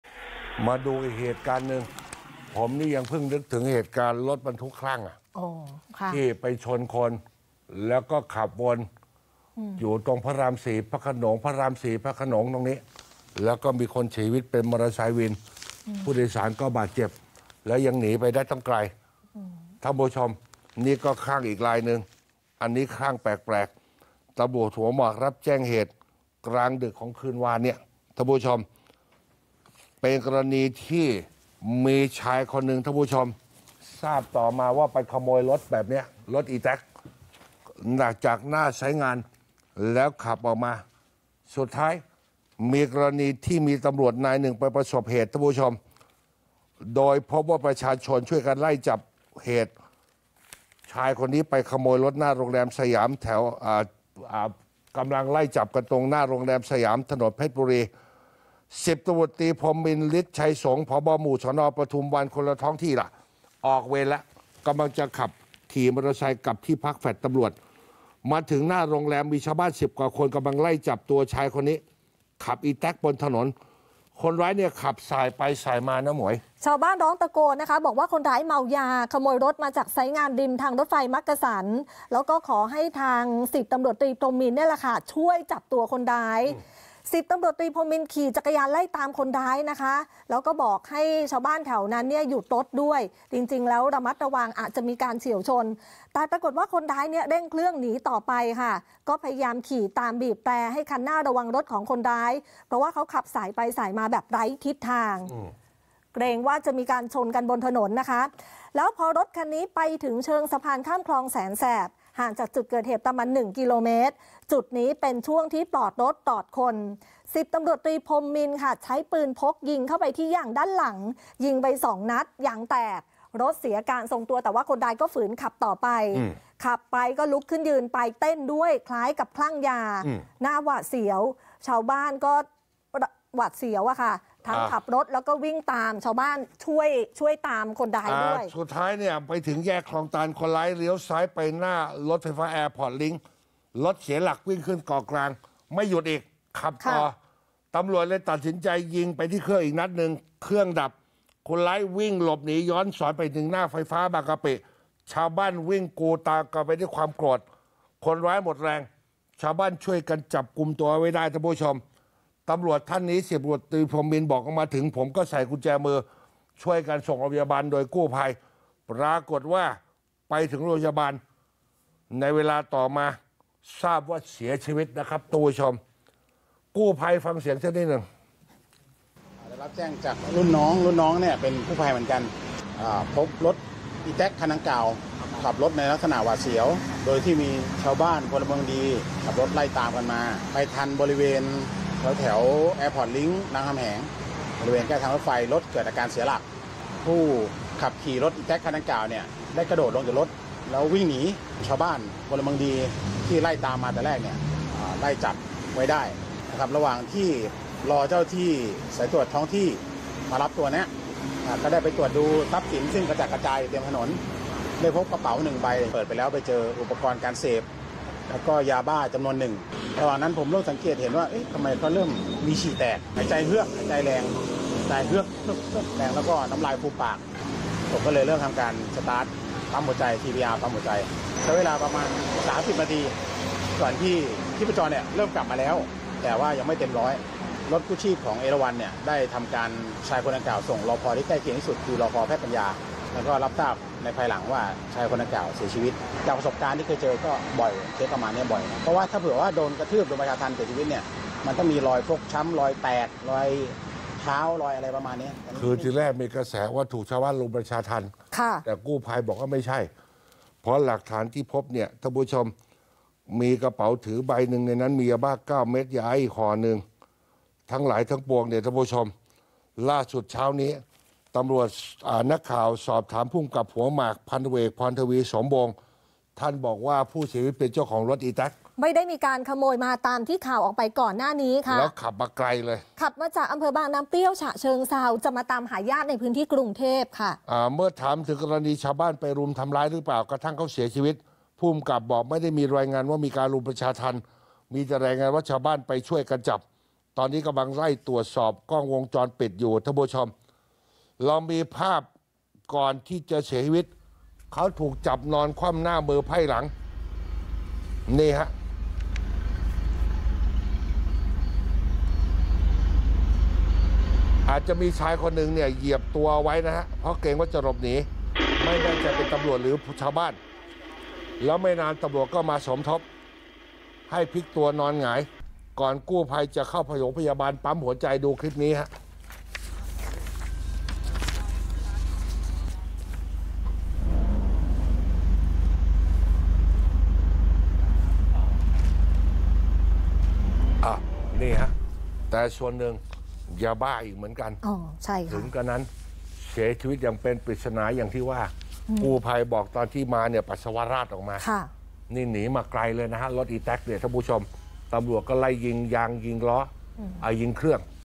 ชายคลั่งยาขโมยรถอีแต๊ก ถูกยิงเครื่องยนต์ดับ ก่อนวิ่งหนีจนหมดแรง ชาวบ้านวิ่งไล่จับ จนพบนอนเสียชีวิต
จากกรณีที่ตำรวจ สน.หัวหมาก รับแจ้งเหตุประชาชนช่วยกันไล่จับคนร้ายที่ขโมยรถอีแต๊ก และขับมาด้วยความหวาดเสียว จากที่พื้นของสน.ปทุมวัน โดยมี ส.ต.ต.พรหมมินทร์ ฤทธิ์ชัยสงค์ ผบ.หมู่(ป) สน.ปทุมวัน ช่วยในการไล่จับมา แต่เมื่อมาถึงเชิงสะพานข้ามคลองแสนแสบ จึงใช้ปืนพกยิงเข้าที่ยางหลังด้านซ้าย 2 นัด จนยางแตก และ ยิงอีกนัดที่ บริเวณแยกคลองตัน จนรถเสียหลักวิ่งขึ้นเกาะกลาง

จากนั้นคนร้ายได้วิ่งหลบหนีไปถึงหน้าการไฟฟ้าบางกะปิ ชาวบ้านที่ตามมาต่างกรูกันวิ่งตามไปด้วยความโกรธแค้น กระทั่งคนร้ายหมดแรง ถูกชาวบ้านจับตัวไว้ได้ และลงประชาทัณฑ์จนคนร้ายแน่นิ่งไป และทราบภายหลังว่าเสียชีวิตที่โรงพยาบาลจากนั้นนำศพไปชันสูตรที่นิติเวชโรงพยาบาลตำรวจ

จากการสอบถาม พ.ต.อ.พรทวี สมวงค์ผกก.สน.หัวหมาก บอกว่าจากการตรวจสอบทราบว่าผู้เสียชีวิตเป็นเจ้าของรถอีแต๋นไม่ได้มีการไปขโมยตามที่เขาออกไปก่อนหน้านี้และได้ขับมาจาก อำเภอบางน้ำเปรี้ยว จังหวัดฉะเชิงเทรา เพื่อมาหาญาติในพื้นที่กรุงเทพมหานคร ส่วนรายละเอียดยังไม่ขอเปิดเผย

และเมื่อถามถึงกรณีที่ชาวบ้านไปรุมทำร้ายจนผู้เสียชีวิตแน่นิ่งไปจะได้รับความผิดหรือไม่ เบื้องต้นผู้กำกับ ระบุว่า ตนไม่ได้รับรายงานว่ามีการรุมประชาทัณฑ์ แต่ได้รับรายงานชาวบ้านไปช่วยไล่จับ

ซึ่งตอนนี้อยู่ระหว่างการตรวจสอบกล้องวงจรปิดและประสานกลุ่มชาวบ้านเข้ามาสอบปากคำ ส่วนตัวผู้เสียชีวิตตอนนี้อยู่ที่ นิติเวชโรงพยาบาลตำรวจต้องรอผลชันสูตรว่าเสียชีวิตจากสาเหตุอะไร เบื้องต้นพบยาเสพติดภายในกระเป๋าของผู้เสียชีวิต